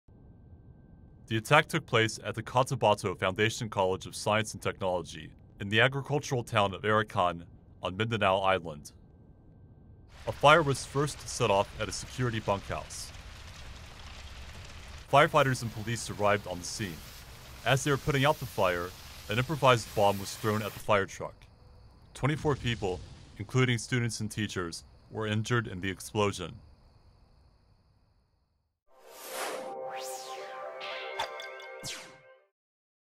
24 injured in Philippines campus blast
At least 24 people were wounded in a bomb attack at the Cotabato Foundation College of Science and Technology in the agricultural town of Arakan on Mindanao Island in the Philippines late Sunday night (January 12).

Firefighters and police were putting out a fire set off at the campus security bunkhouse when an improvised explosive device was thrown at a fire truck, causing a blast that injured at least 24 people.

"The fire drew responders, including firemen, teachers, and students, and they were among the injured," Senior Inspector Rolly Oranza of the local police force said in an AFP report. Seventeen of the injured were in critical condition.

Investigators have not yet identified an attacker and are still trying to determine whether the fire was set to lure in authorities before the explosion, Oranza said.

In September 2013 Arakan police identified and defused a bomb planted near the school's flagpole, Oranza said.

Mindanao is mostly populated by indigenous tribes. Although Islamist rebels are not known to operate in Arakan, a farming community of 45,000 people, an Islamist insurgency throughout Mindanao has killed more than 150,000 people.